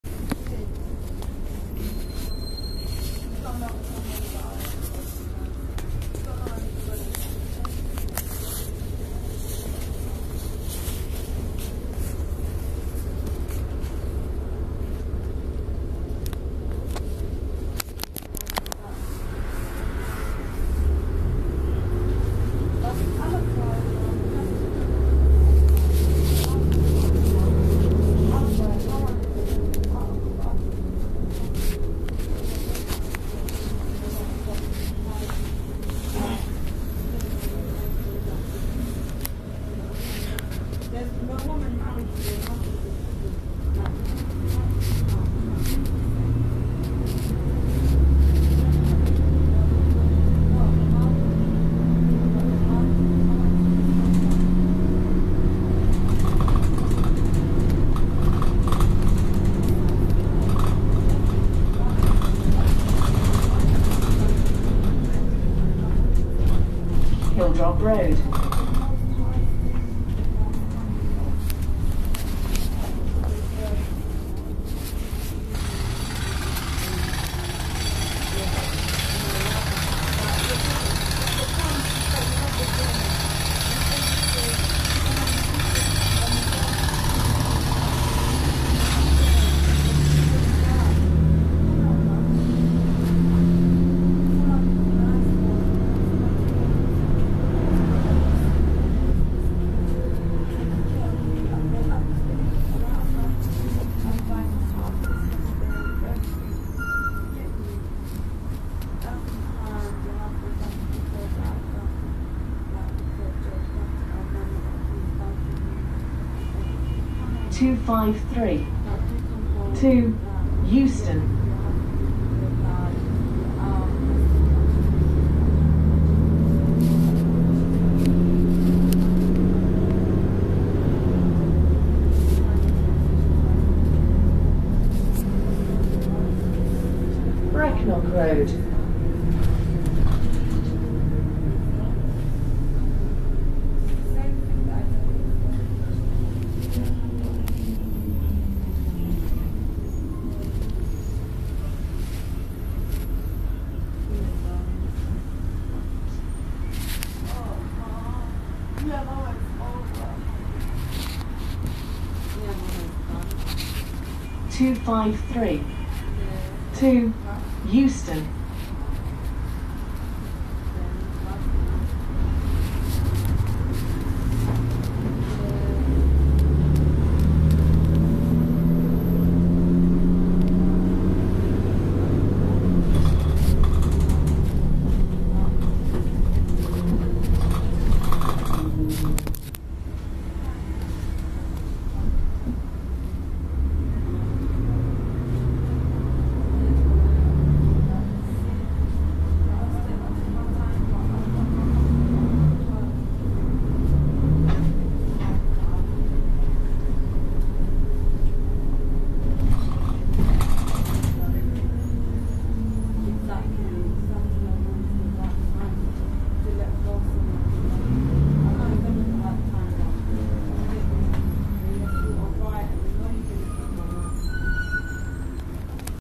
Arriva London Alexander Dennis (DAF, DB250) T298FGN DLA98
This bus was serving Route 253 to Euston

Route 253 Hackney Central - Euston
Alexander Dennis (DAF, DB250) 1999 T298FGN DLA98
Operated by London Arriva London (Stamford Hill Garage, SF)
Thursday 18th March 2010